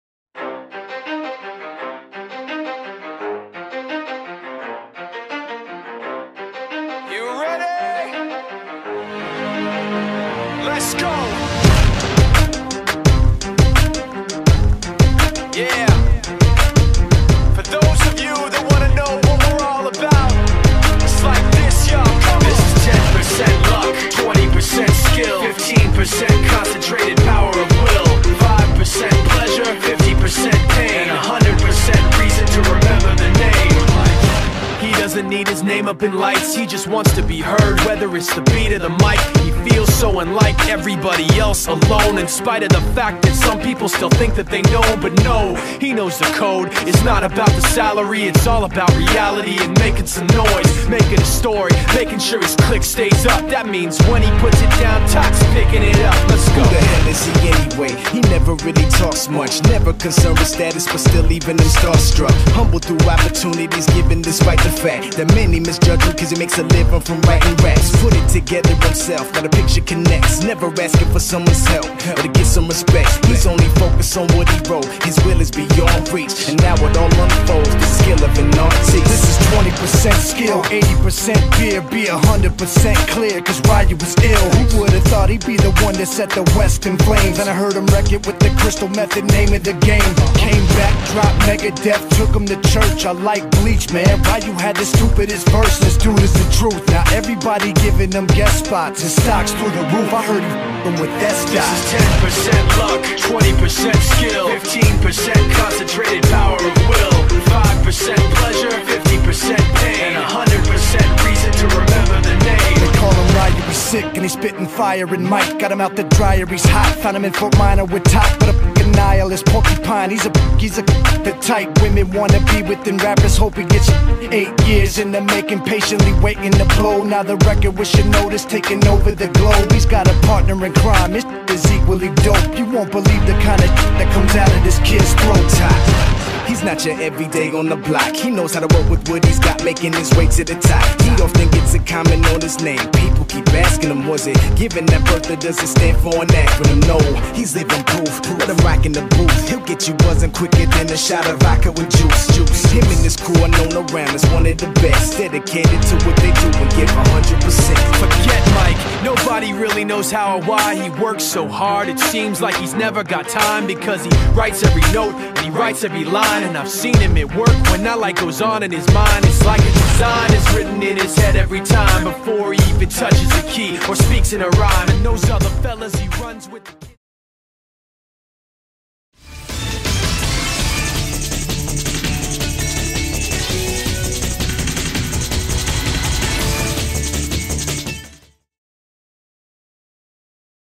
SCSU vs Fairfield University Hockey Highlights
The Owls of Southern Connecticut hosted the Stags of Fairfield University, at the Louis Astorino Ice Arena in Hamden Connecticut. Witness the highlights from the Home Opener, hard hitting extravaganza on ice.